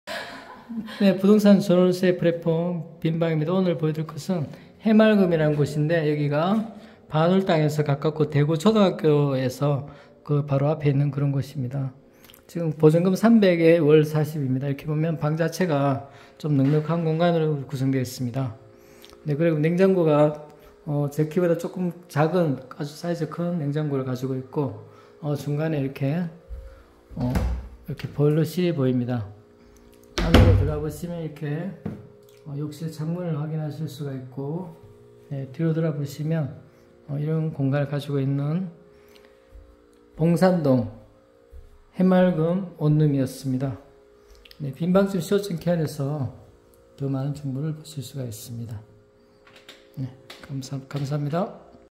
봉산동 반월당 역 인근 해맑음 원룸 300/40
에서 "해맑음"으로 검색해 보세요.

정보
위치 대구광역시 중구 봉산동 58-33번지
도로명 대구광역시 중구 봉산문화2길 8-9
동/호수 304
건물정보
해당 3층전체 4층
상세정보
공급면적 25㎡ (7.56평)
방향 남동기준 안방
난방 도시가스입주일 즉시
주용도 단독주택
기타용도 다가구주택
층수 지상4층/지하0층
승강기수 0기
구조 철근콘크리트구조
세대/가구 0세대/ 10가구
대지면적 155.7㎡
연면적 267.35㎡ (용적율 171.71%)
건축면적 91.14㎡ (건폐율 58.54%)
주차 옥외 0대, 옥내 2대
사용승인일 20140919
건축물대장 층별개요
주용도 층 면적
다가구주택 지상 3층 91.14㎡
다가구주택 지상 1층 12.96㎡
다가구주택 지상 2층 91.14㎡
다가구주택 지상 4층 72.11㎡

대구광역시 북구 복현로 32-1 캠퍼스부동산중개사무소 | 대표자: 김성택
사업자등록번호: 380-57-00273 | 부동산등록번호: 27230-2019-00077